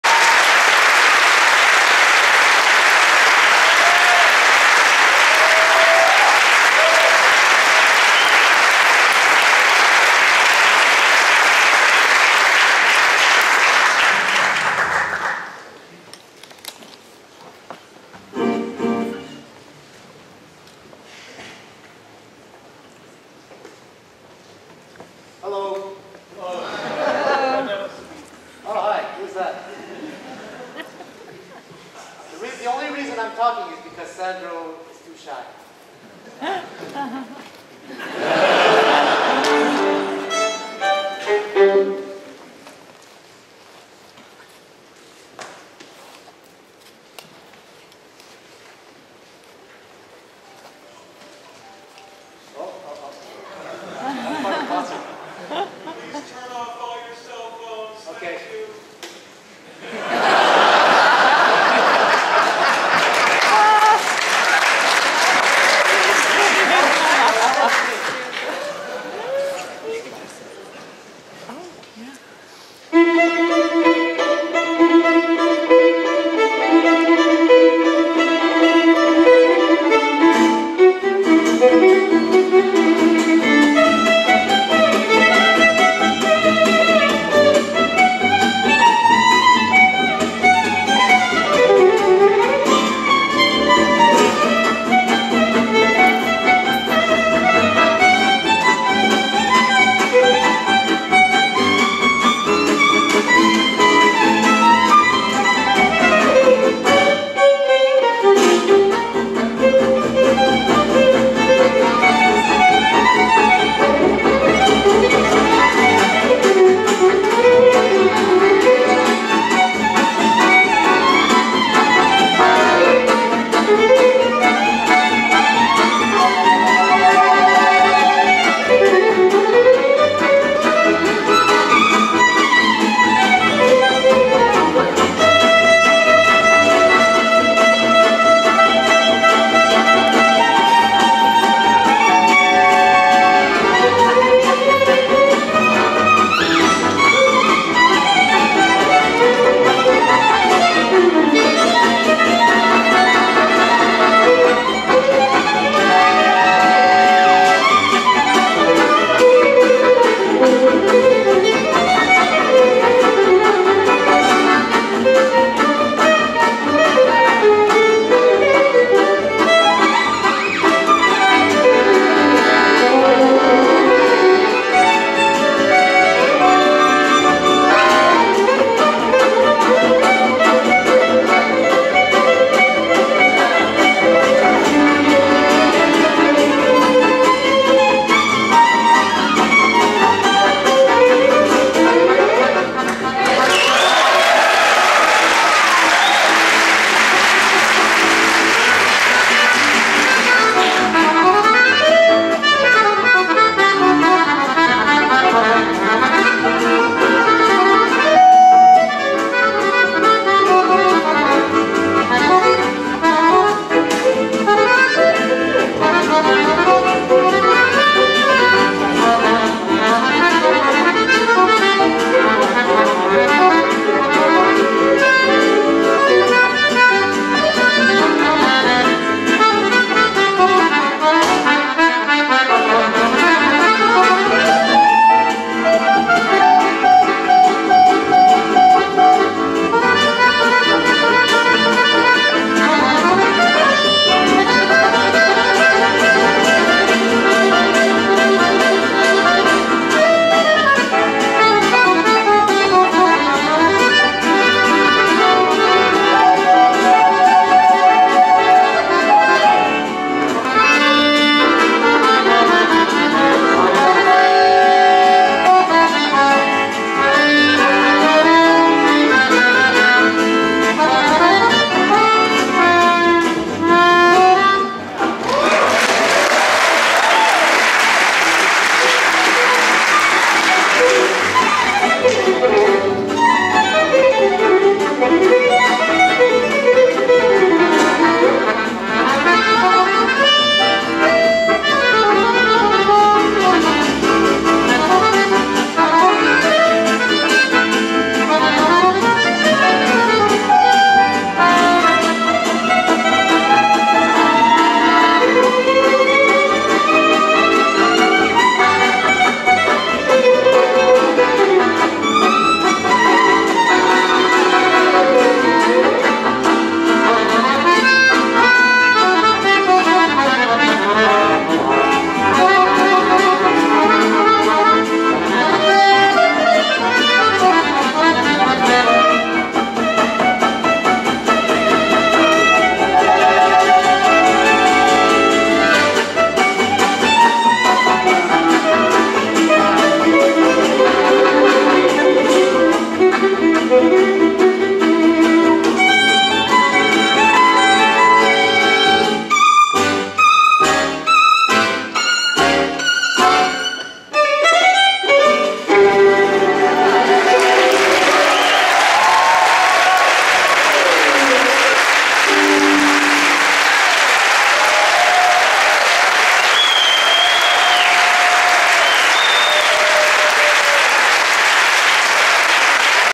Django in June 2015 - Sandro Roy and friends.
Django in June-2015
Violin: Sandro. Roy
Double Bass: Ghali Hadefi
Guitar: Denis Chang-Guitar,
Acordeon: ? (need help pls)